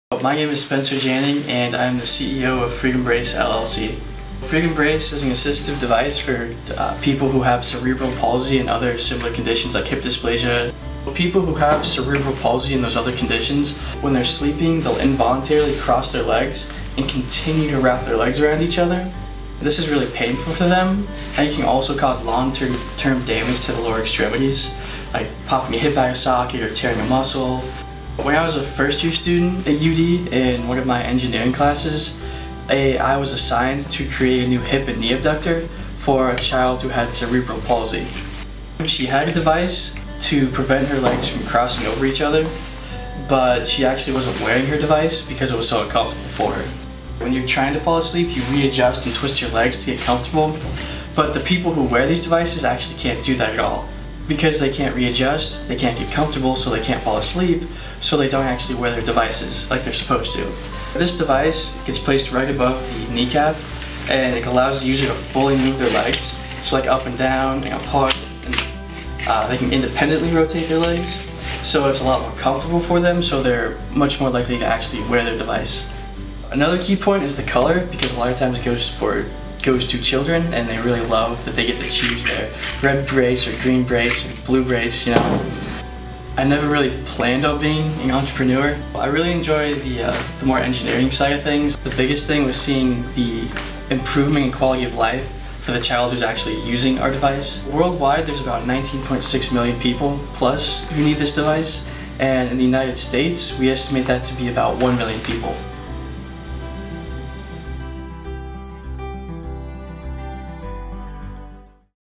Spencer Janning, Freedom Brace
Spencer Janning was assigned a project in an engineering class — develop a new, more comfortable brace for a child with cerebral palsy. Fast forward — the University of Dayton student has launched his own company, Freedom Brace.